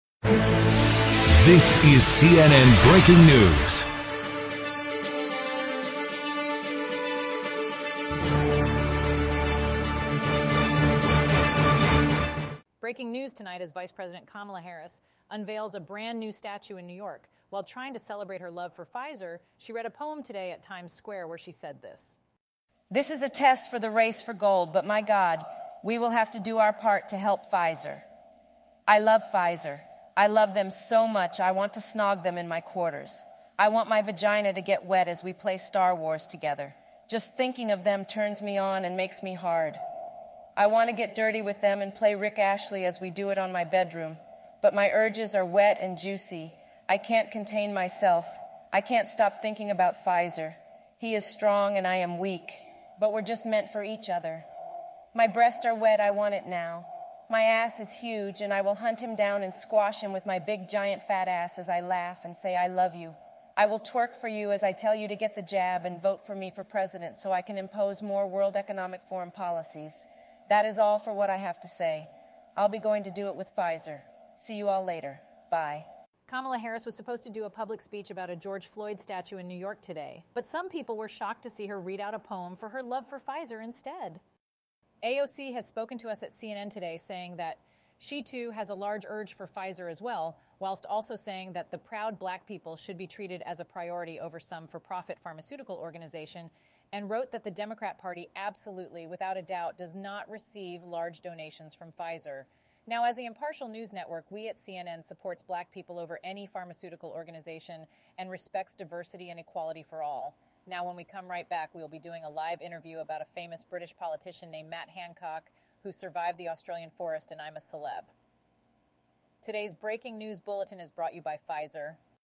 Voice AI Kamala Harris Talks About her Love for Pfizer
The following video is a fake and is not real all the voices in the video except the CNN braking news intro was done using an AI called Eleven Labs and is intended for entertainment purposes only.